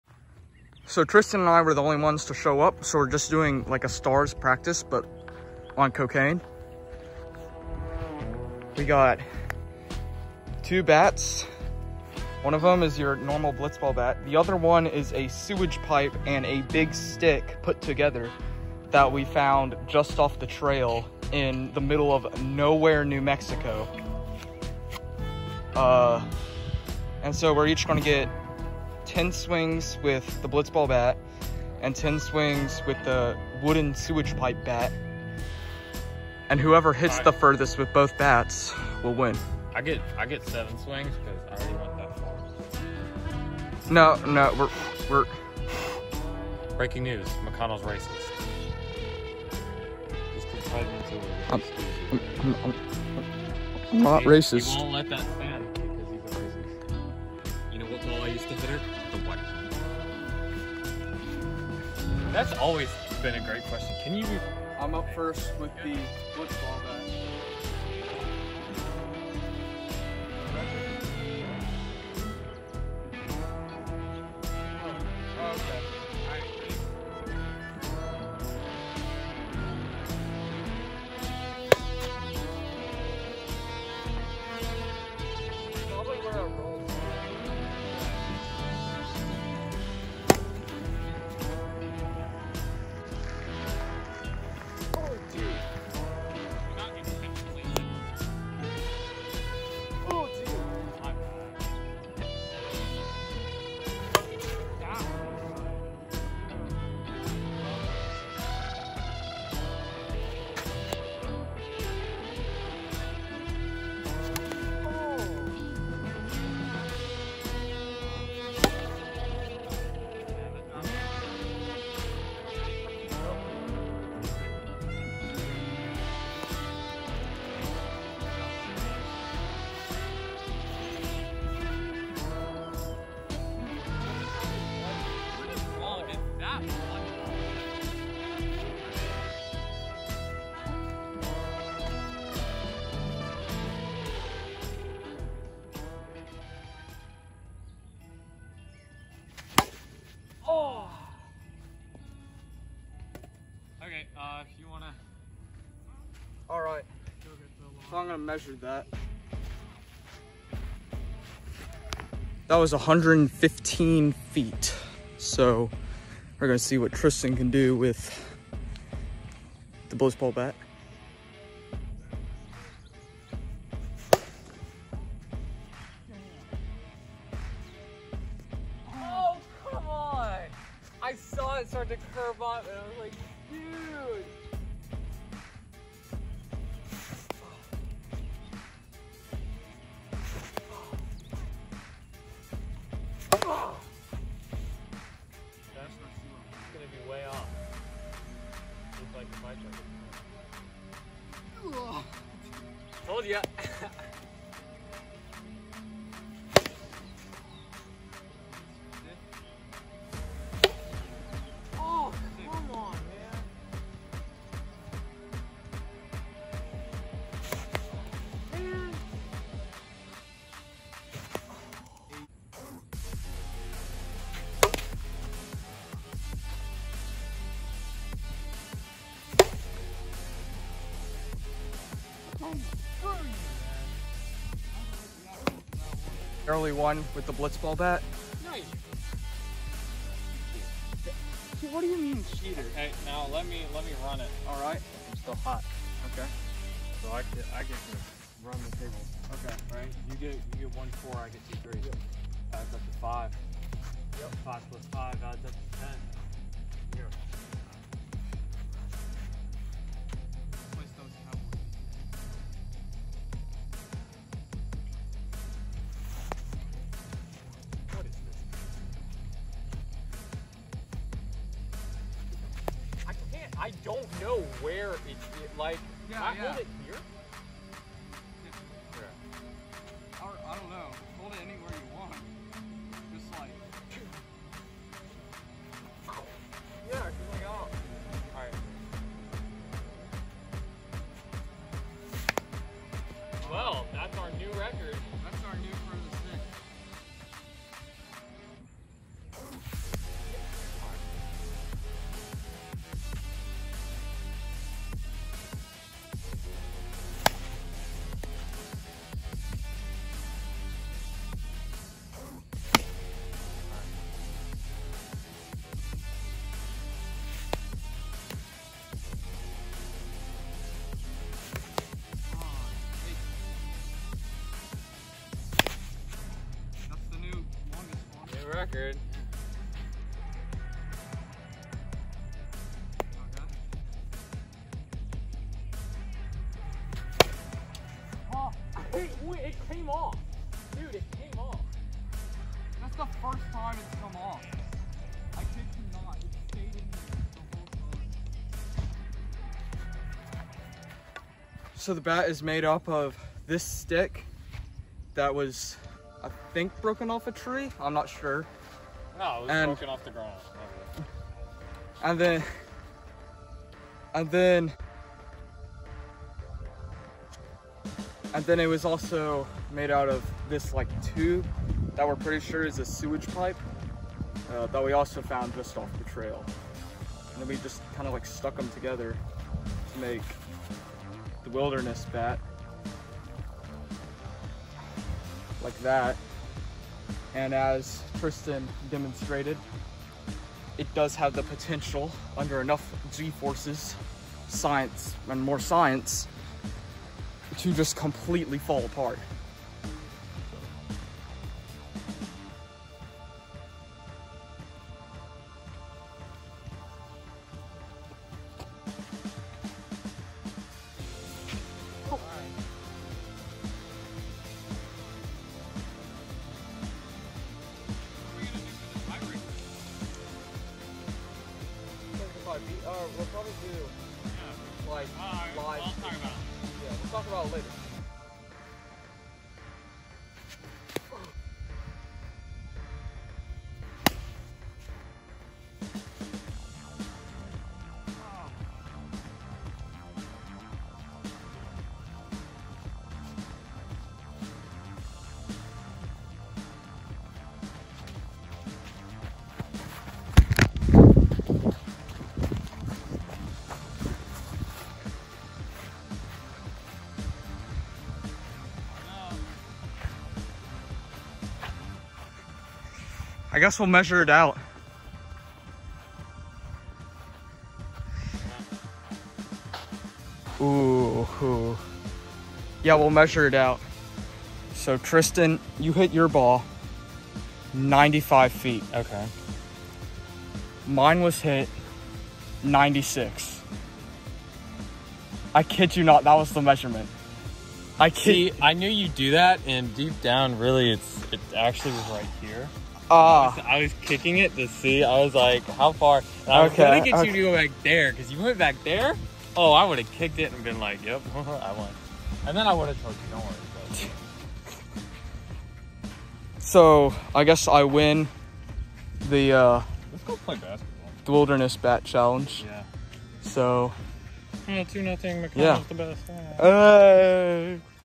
“Wilderness Bat” Challenge
McConnell and Tristan tee off in a Star v Star matchup to try to hit the farthest homer with both a blitzball bat, as well as a crude 2 minute craft.